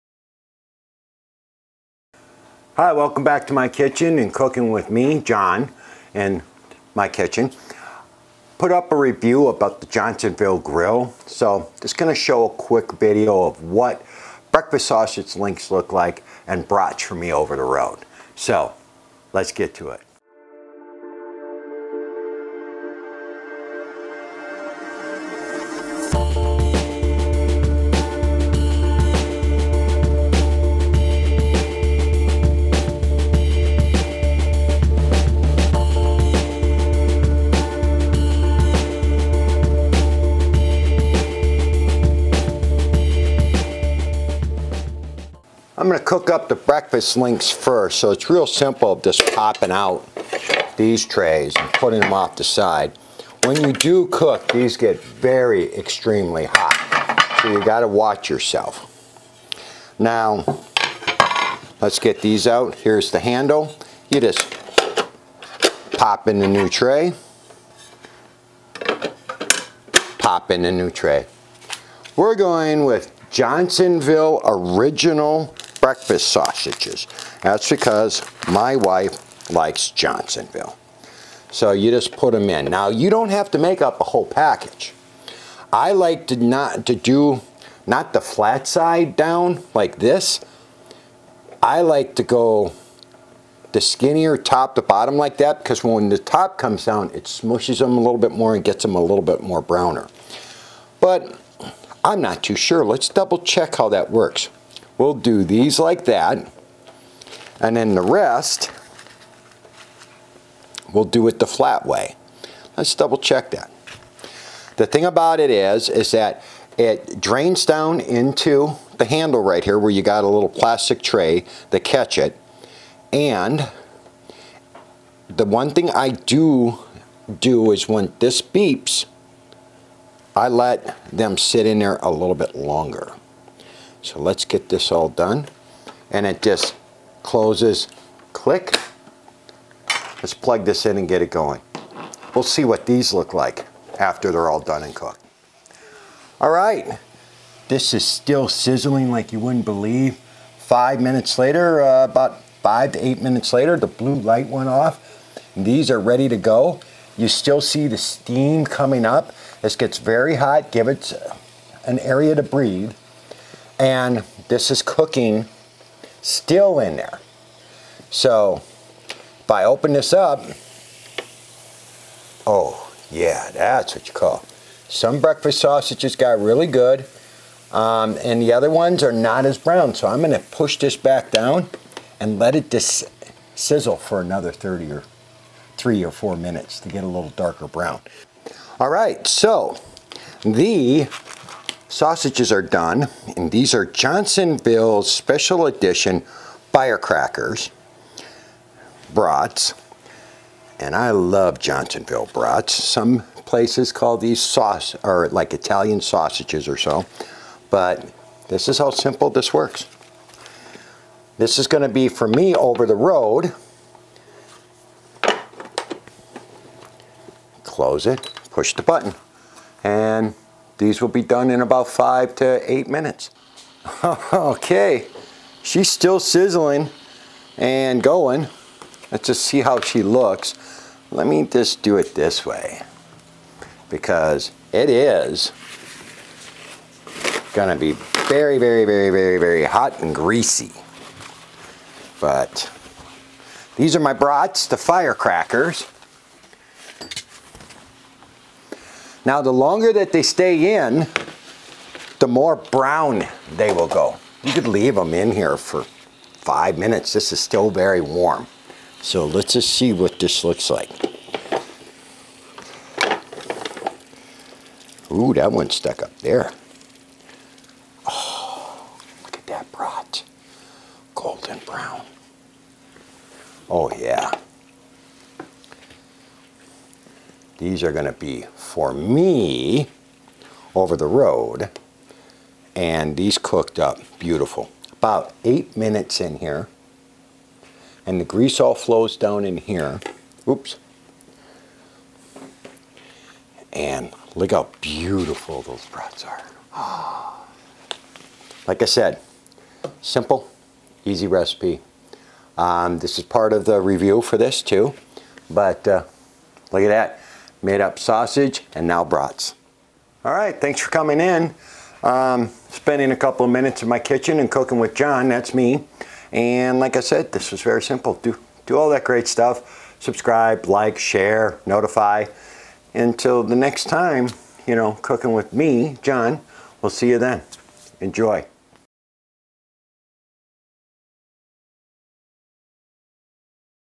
Johnsonville Sizzling Grill
Making perfect Brats, Breakfast Links or Patties, Johnsonville Grill 3 in 1 can make it easy. A cold day or raining out , just grill them inside with the outside flavor.

I didn't make up a recipe card for this - easy too do.

Show #11